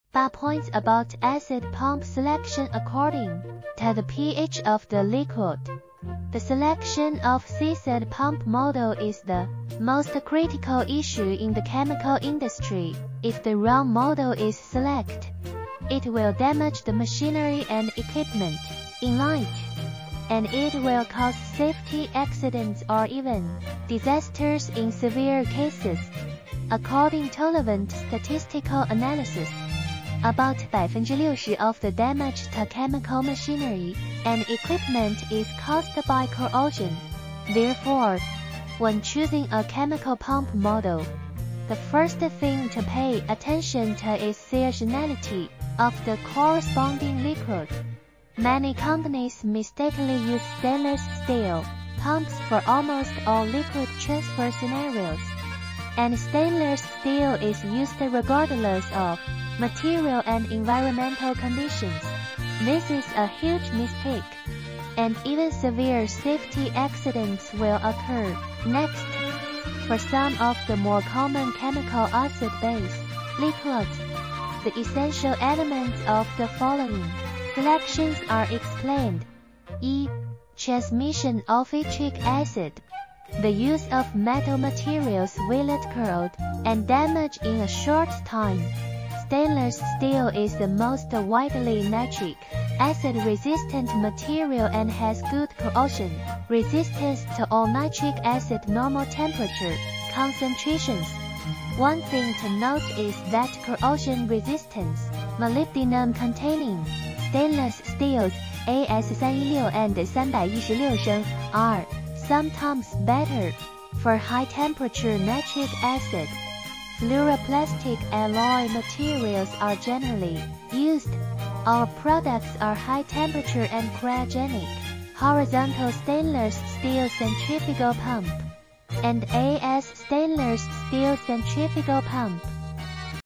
8 Points About Acid Pump Selection According to the PH of the Liquid
Choice of Acid Pump
The selection of the acid pump model is the most critical issue in the chemical industry. If the wrong model is select, It will damage the machinery and equipment in light, and It will cause safety accidents or even disasters in severe cases.

According to relevant statistical analysis, about 60% of the damage to chemical machinery and equipment is caused by corrosion. Therefore, when choosing a chemical pump model, the first thing to pay attention to is the rationality of the corresponding liquid.

Many companies mistakenly use stainless steel pumps for almost all liquid transfer scenarios, and stainless steel is used regardless of material and environmental conditions. This is a huge mistake, and even severe safety accidents will occur. Next, for some of the more common chemical acid-base liquids, the essential elements of the following selections are explained:

Centrifugal Pump

1. Transmission of nitric acid: The use of metal materials will not corrode and damage in a short time. Stainless steel is the most widely nitric acid-resistant material and has good corrosion resistance to all nitric acid Normal temperature concentrations. One thing to note is that corrosion resistance molybdenum-containing stainless steels (AS316 and 316L) are sometimes better. For high-temperature nitric acid, fluoroplastic alloy materials are generally used. Our products are High Temperature and Cryogenic Horizontal Stainless Steel Centrifugal Pump, AS Stainless Steel Centrifugal Pump.

PVDF Pump

2. Acetic acid transmission: Acetic acid is one of the most corrosive substances in organic acids. It will severely erode general metal materials in acetic acid of any concentration value and temperature. Stainless steel is an excellent sour acid-resistant material. Molybdenum-containing 316 stainless steel also Can be applied to high temperature and dilute acetic acid steam. High-alloy stainless steel or fluoroplastic pumps can be used for harsh requirements such as high-temperature, high-concentration acetic acid, or other corrosive substances. For this acid, our targeted products are MD PVDF magnetic drive pump, MDH PVDF magnetic drive pump, BT PVDF vertical pump, PTH PVDF vertical pump, PD PVDF self-priming pump.

long-shaft horizontal centrifugal pump

3. Transfer alkali: alkaline water (sodium hydroxide) is generally not very corrosive, but alkali solutions usually form crystals. You can use a long-shaft horizontal centrifugal pump equipped with a mechanical seal made of siliconized graphite 169.

PP pump

4. Transporting ammonia water: Most ammonia (ammonium hydroxide) is slightly corroded in liquid ammonia and ammonia (ammonium hydroxide). In addition, you can not use copper materials. Most DUBAO products are ideal for the transportation of ammonia and ammonia water. Therefore: MD PP magnetic pump, MDH PP magnetic pump, PD PP Plastic self-priming pump are the most suitable.

5. Transfer salt water: Saltwater (Seawater) usually has a relatively low corrosion rate of sodium chloride solution, seawater, and saltwater and is generally protected by coatings; stainless steel corrosion rate is shallow, but chloride ions may cause local corrosion, typically 316 stainless steel. Various chemical pumps are available in 316 material configurations, such as SUS MD magnetic drive pumps.

Sealless Magnetic Pumps

7. As one of the corrosive solid substances, sulfuric acid is an important industrial raw material widely used. The concentration of concentrated sulfuric acid is above 80 °C, and the temperature is below. One thing to note, esters and ethers are soluble at 80°C. High carbon steel and cast iron have better corrosion resistance, but they are not suitable for high-speed flowing sulfuric acid and are not ideal for pump valve materials. Generally, stainless steel such as 304 (Cr18Ni9) and 316 (0Cr18Ni12Mo2Ti) is also limited. Therefore, the sulfuric acid pump is usually made of high-silicon cast iron (difficult to process) and high-alloy stainless steel (20 alloys). Still, it isn't easy to operate and expensive, and people do not favor it. The fluoroplastic alloy centrifugal pump has good sulfuric acid resistance. The applicable products are MD sulfuric acid magnetic drive pump, MDH sulfuric acid magnetic drive pump, and PTH MD sulfuric acid vertical pump.

HCL Pump

8. Most metal materials are resistant to hydrochloric acid corrosion (including stainless steel materials), and molybdenum-containing ferrosilicon can only be used at 50°C and below 30%. Unlike metal materials, most non-metallic materials have good corrosion resistance to hydrochloric acid, so lined rubber pumps and plastic pumps (such as engineering plastics, fluoroplastics, etc.) are the best choices for delivering hydrochloric acid. Applicable products are MDH hydrochloric acid magnetic drive pumps and MD HCL magnetic drive pumps.